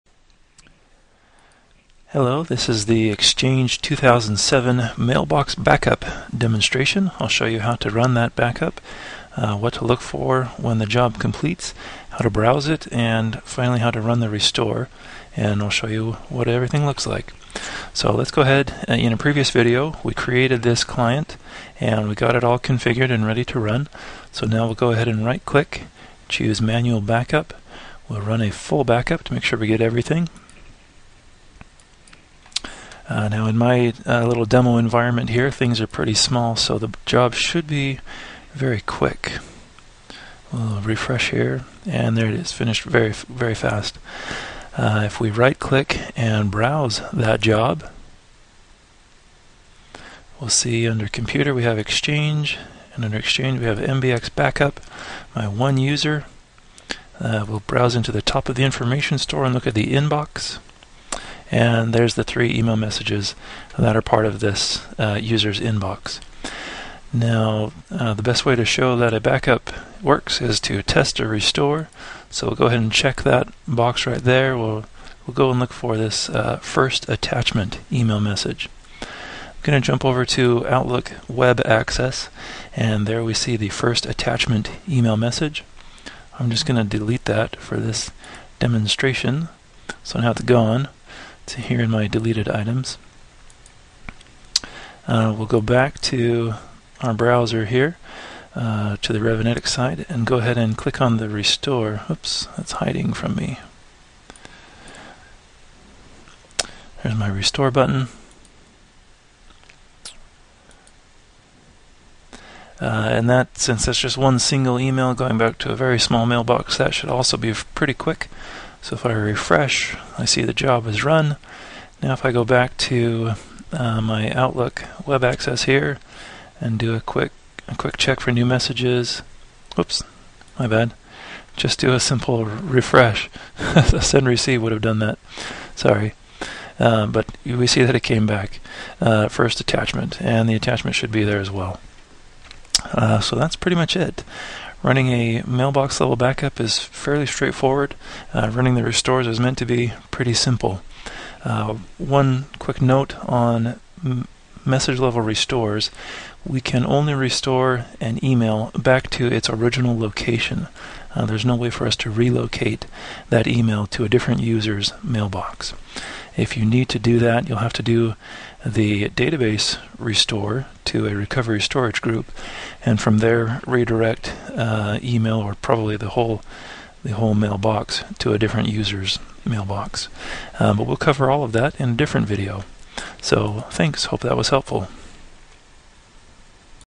Revinetix - Exchange 2007 Mailbox Backup and Restore
How to use the Revinetix RevOS solution to backup and restore an Exchange 2007 mailbox or email message.